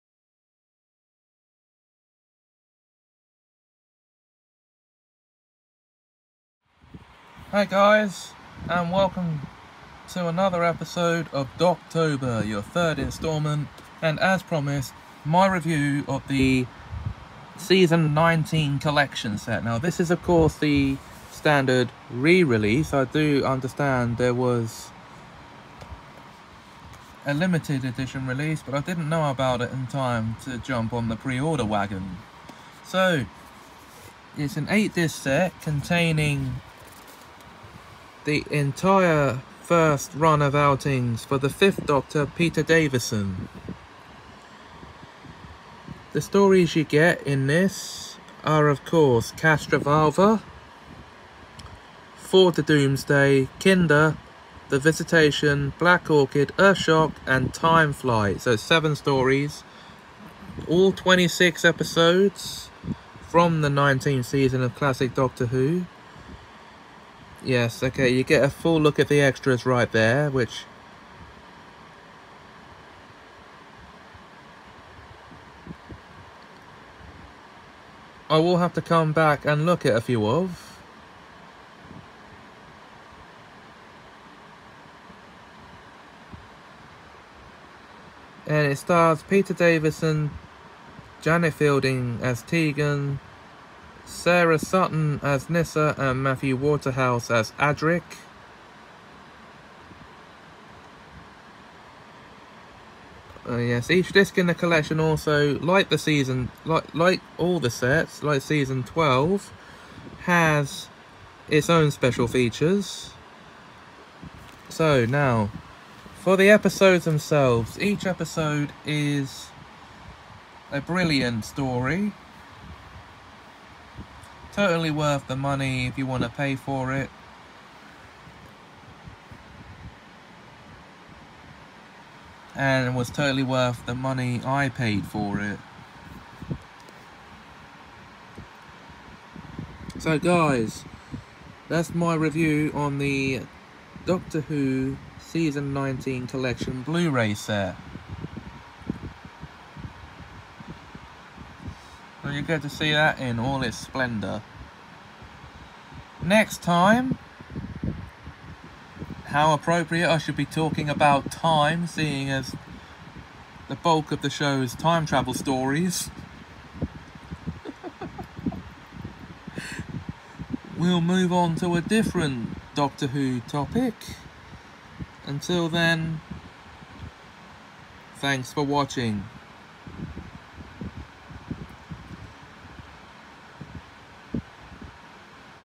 Doctober 003: Season 19 Collection Blu-ray Review
I review the 2021 standard re-release of the Season 19 Collection Blu-ray set